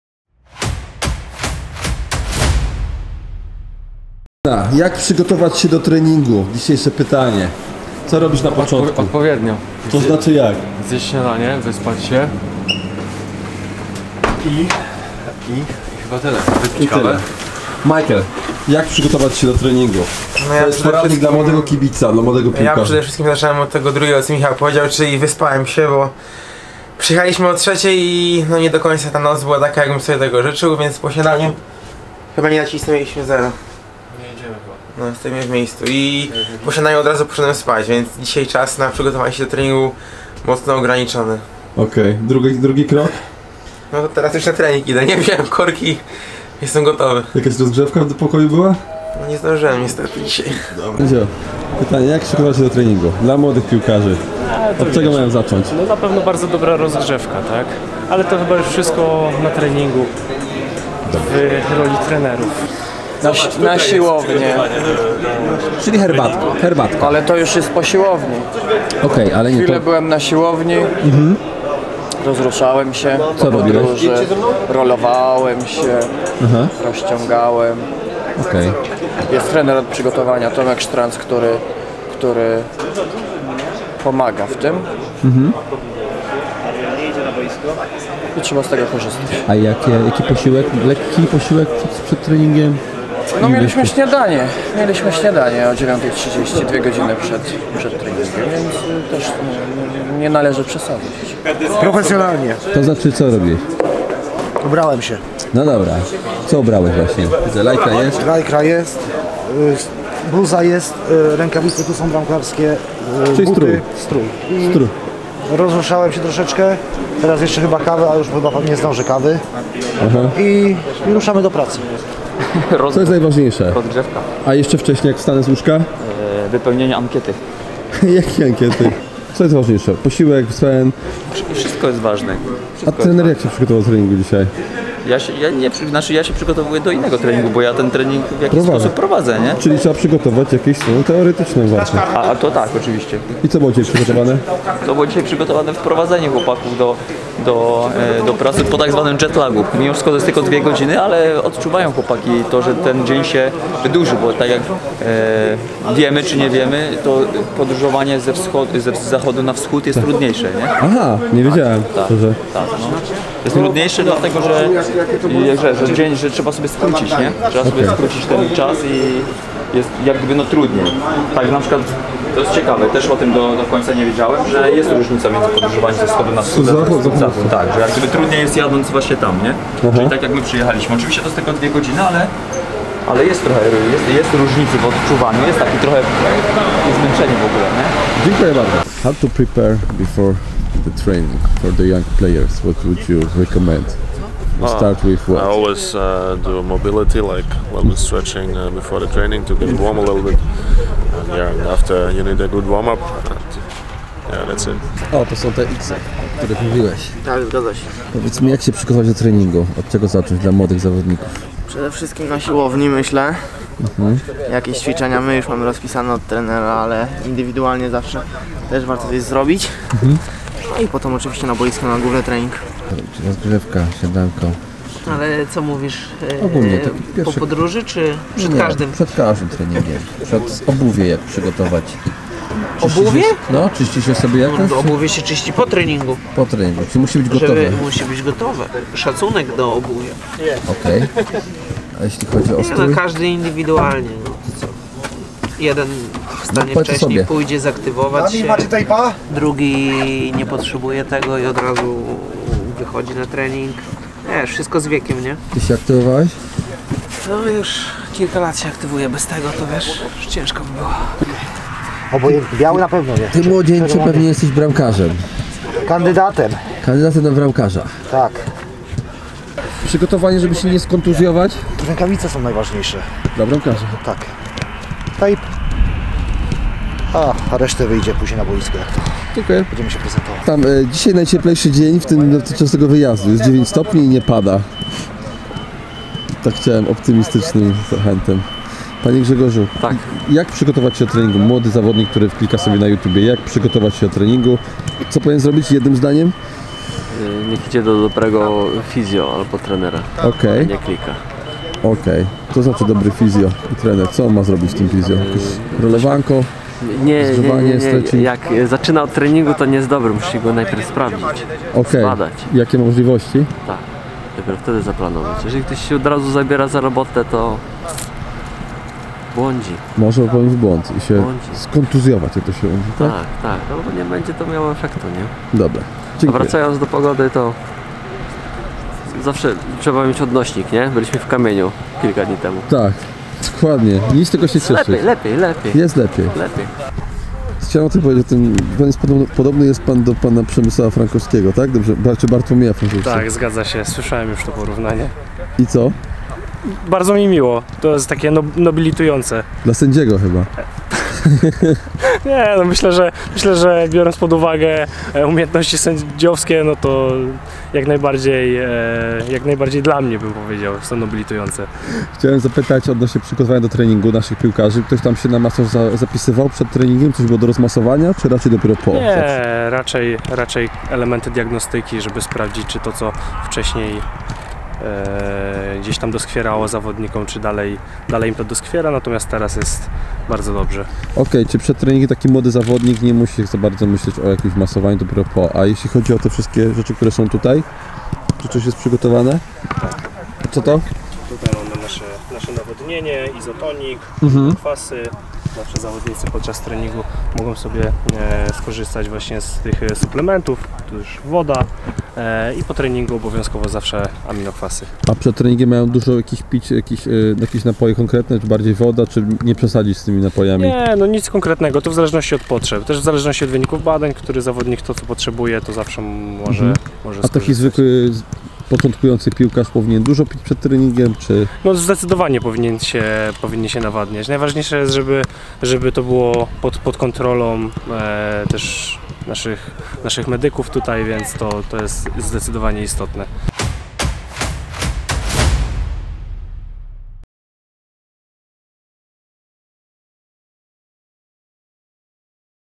JAK PRZYGOTOWAĆ SIĘ DO TRENINGU PIŁKI NOŻNEJ?
Czyli co chcecie wiedzieć na temat przygotowania do treningu piłki nożnej.